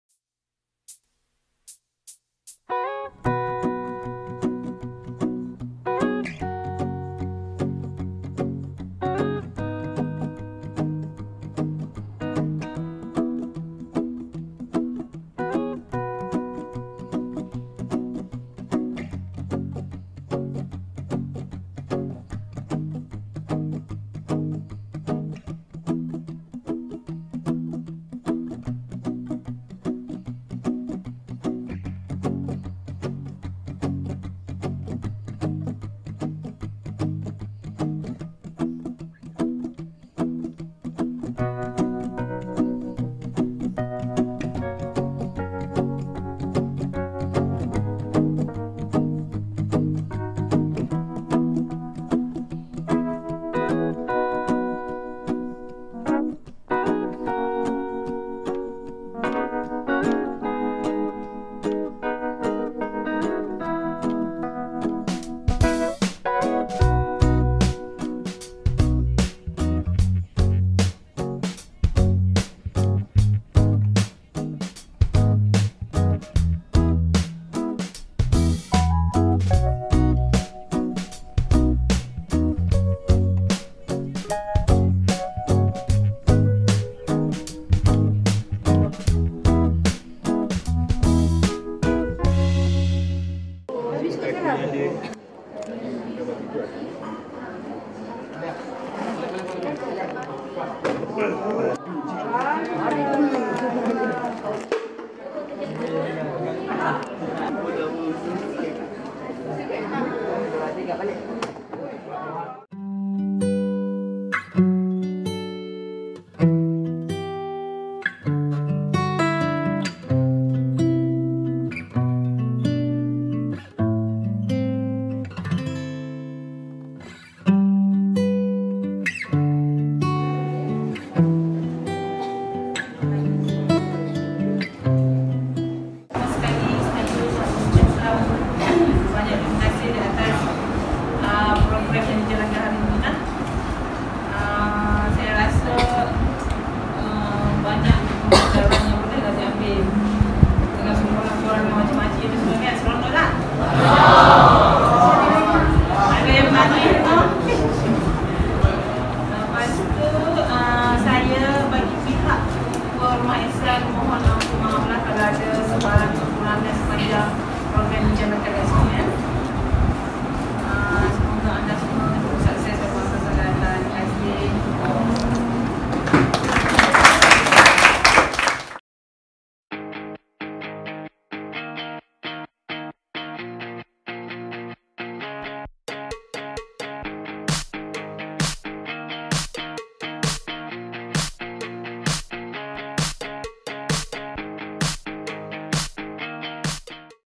CPR2011 KUMPULAN 1 (GROUP A - 2019)
Hasil daripada pelaksanaan aktiviti bersama-sama penghuni Rumah Ehsan Kuala Kubu Bharu.